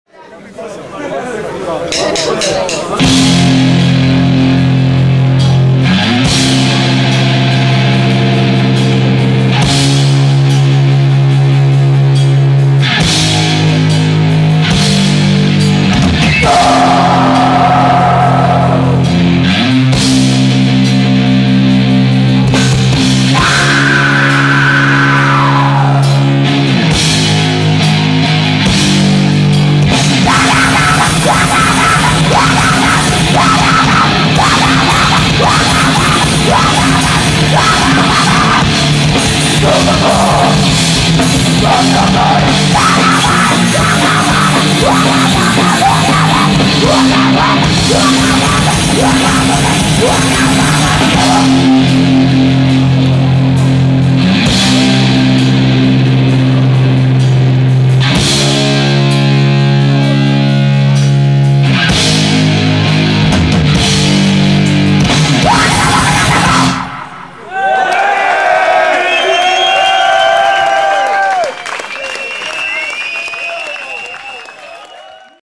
Idiot´s Parade(sk)
good fast band from Slovakia guitar, female singer,drums and total laxage